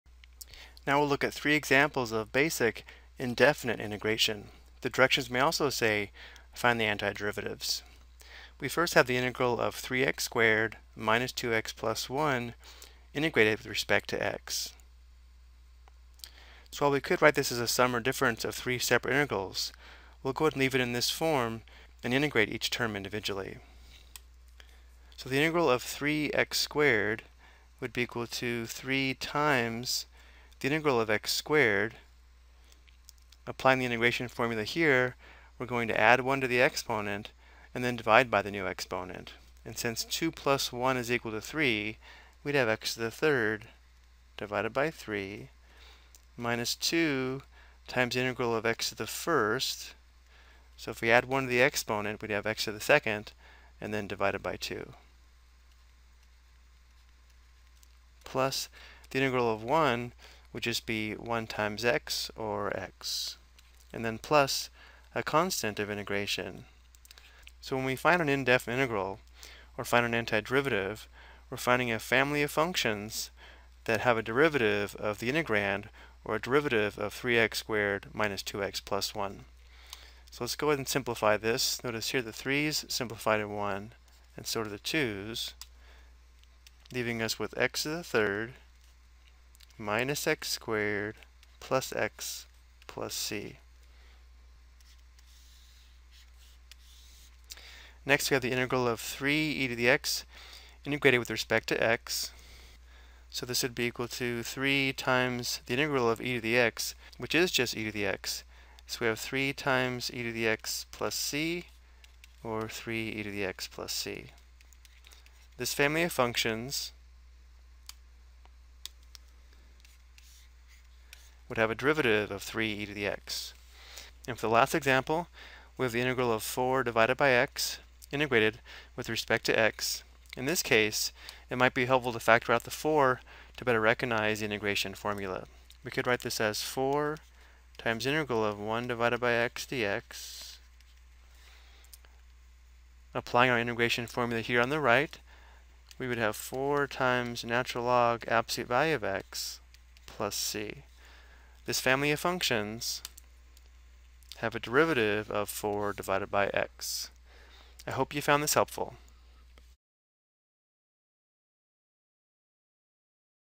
Ex: Basic Indefinite Integration (Polynomial, Exponential, Quotient)
This video provides three basic examples of how to determine antiderivatives using integration formulas.